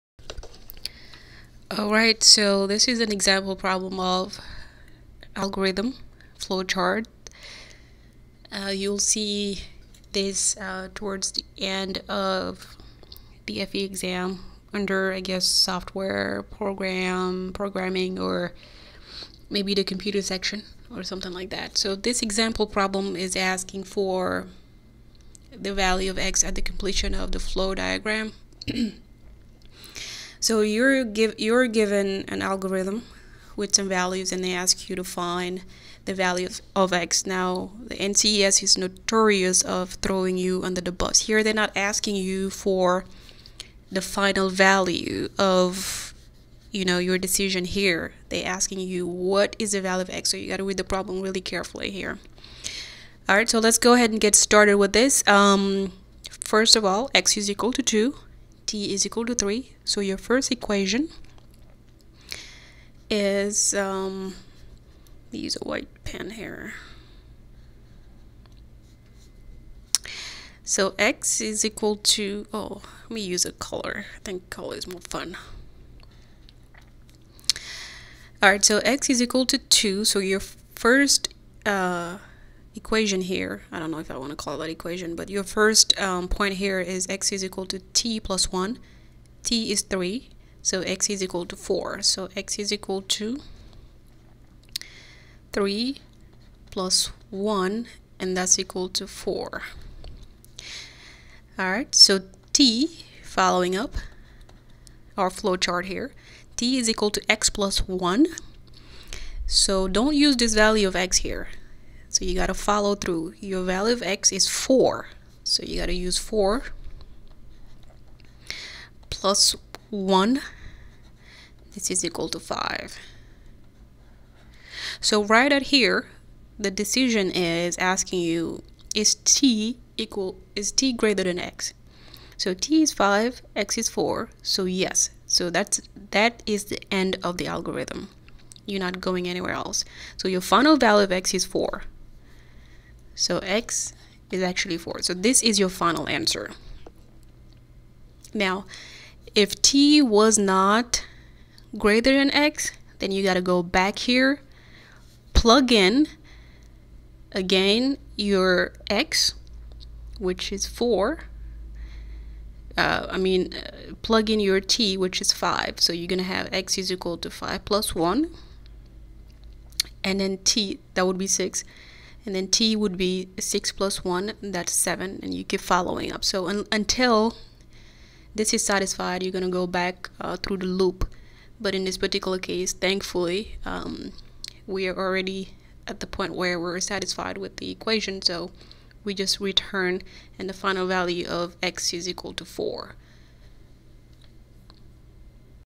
Algorithm Flowchart--FE Review
Example problem on algorithm flowchart. Find the value at the end of the flowchart.